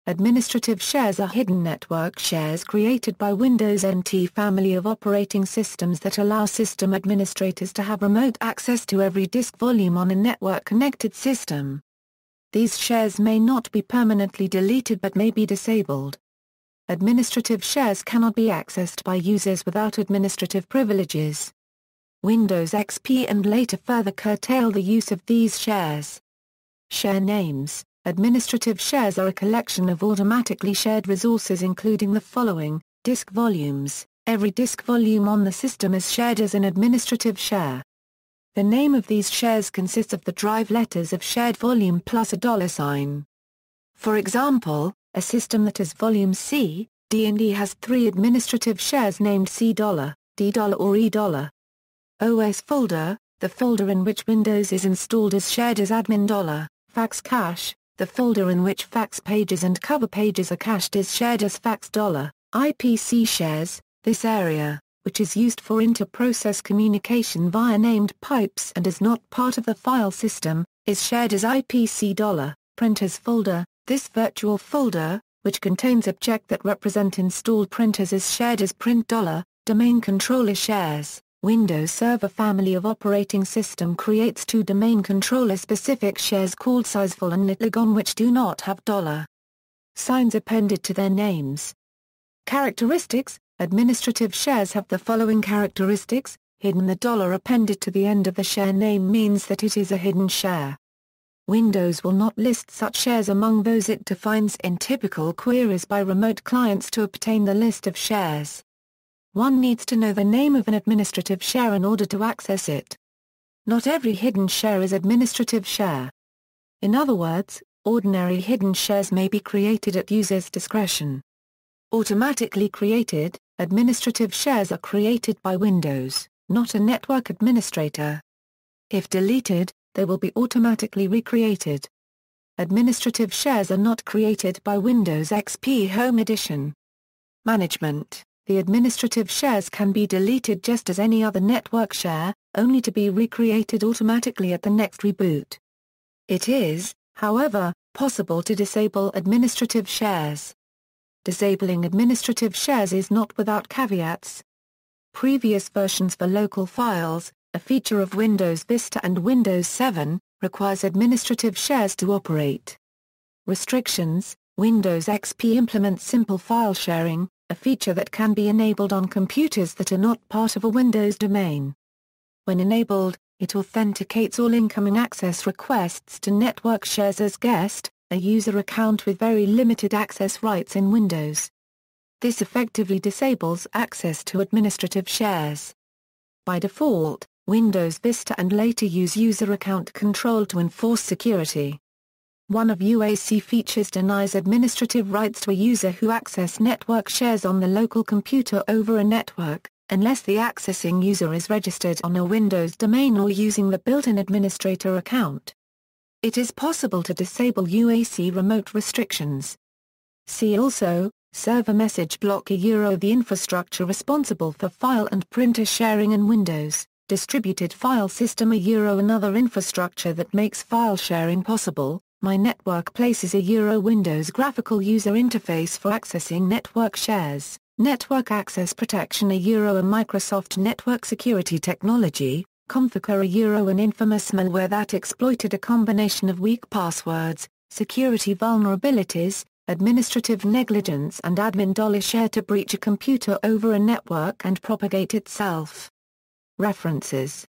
Administrative share
Administrative shares are hidden network shares created by Windows NT family of operating systems that allow system administrators to have remote access to every disk volume on a network-connected system. These shares may not be permanently deleted but may be disabled. Administrative shares cannot be accessed by users without administrative privileges. Windows XP and later further curtail the use of these shares.